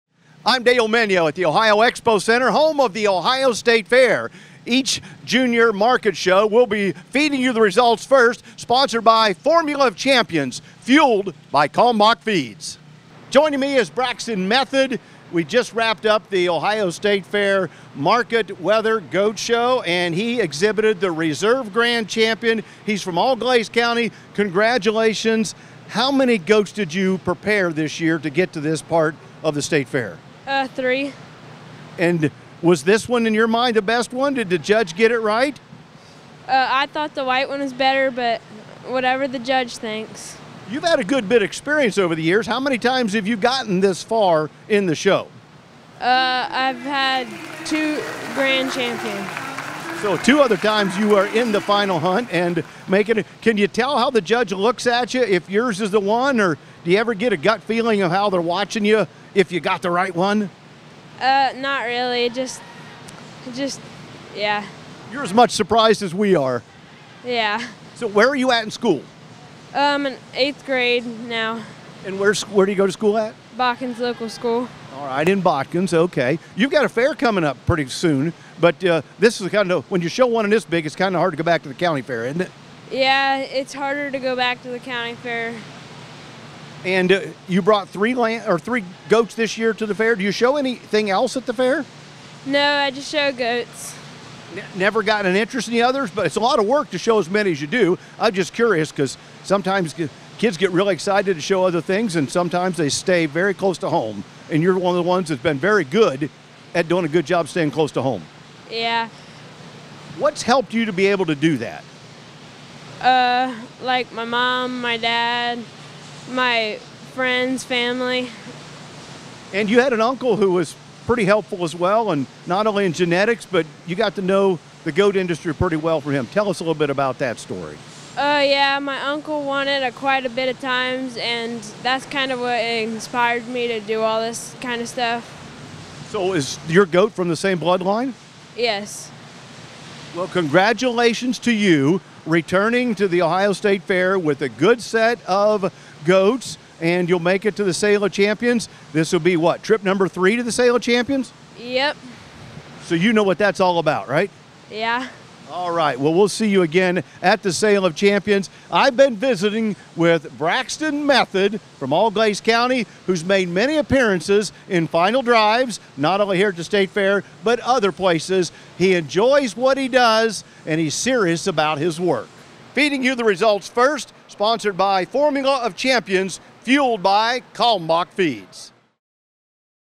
2024 Ohio State Fair Reserve Grand Champion Market Wether - Braxton Method, Auglaize County
Braxton Method is no stranger to the Sale of Champions at the Ohio State Fair, and will be back again this year with his Reserve Grand Champion Market Wether. In this video, Ohio Ag Net's Dale Minyo catches up with Braxton immediately after the show.